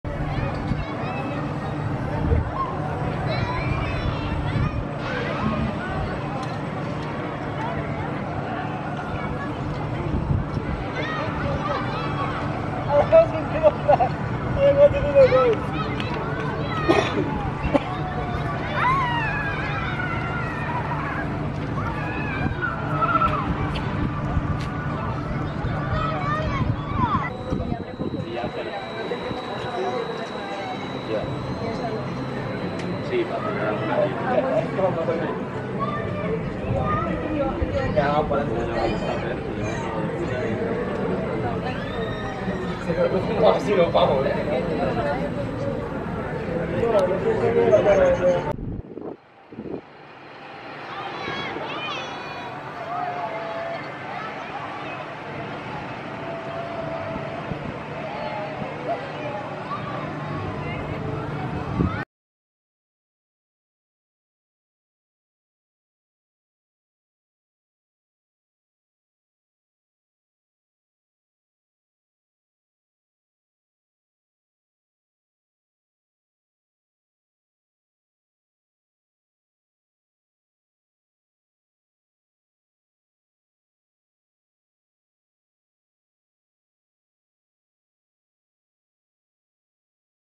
Star Flyer Off-Ride Footage, Parque de Atracciones de Madrid Funtime Swing Ride | Non-Copyright
Star Flyer is the tallest ride at Parque de Atracciones de Madrid.  This is a 26 story tall swing ride from Funtime.  While it's slower than other star flyers, it offers a spectacular view of the park, mountains, and city.